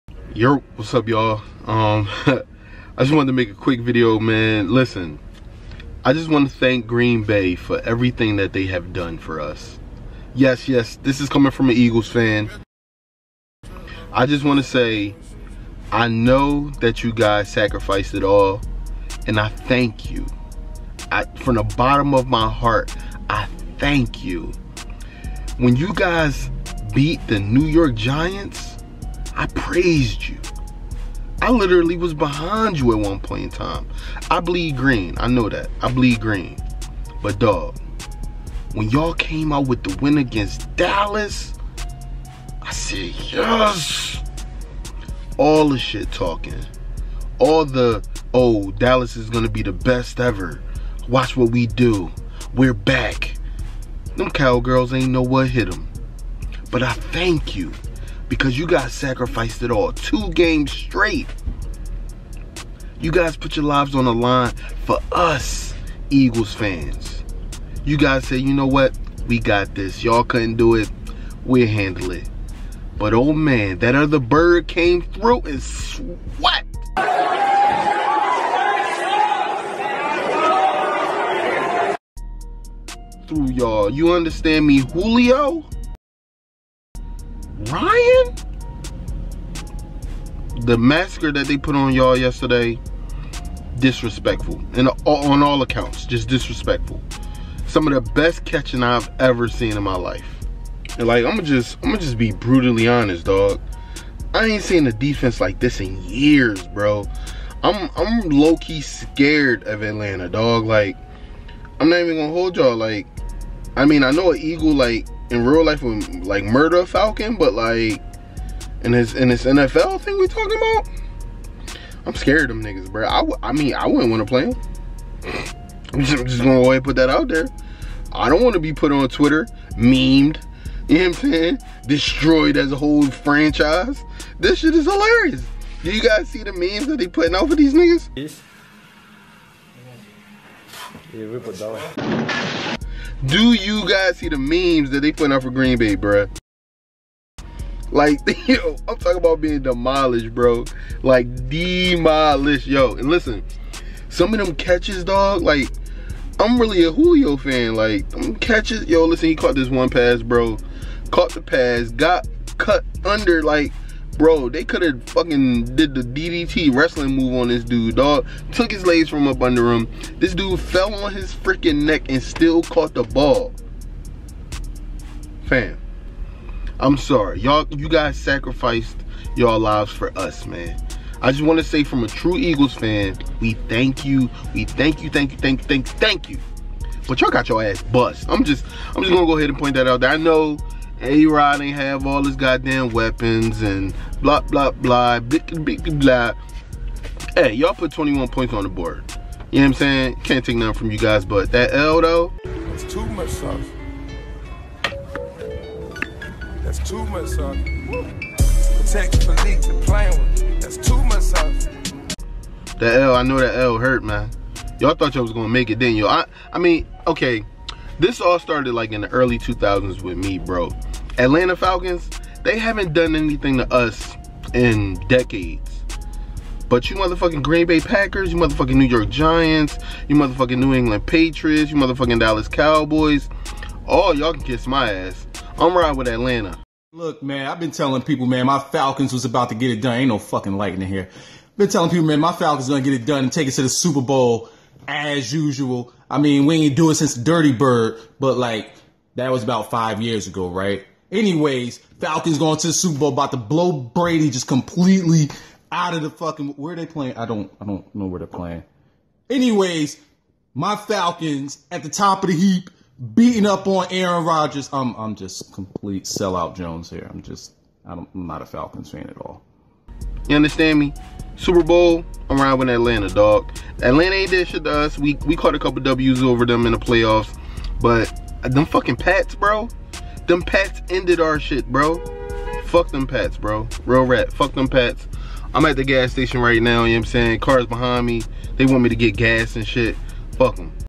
THANK YOU GREEN BAY PACKERS! YOUR SACRIFICE IN ATLANTA WON'T BE FORGOTTEN... FROM AN EAGLES FAN
THANK YOU GREEN BAY! YOUR SACRIFICE IN ATLANTA WON'T BE FORGOTTEN... FROM AN EAGLES FAN

My Recording Equipment For This Video!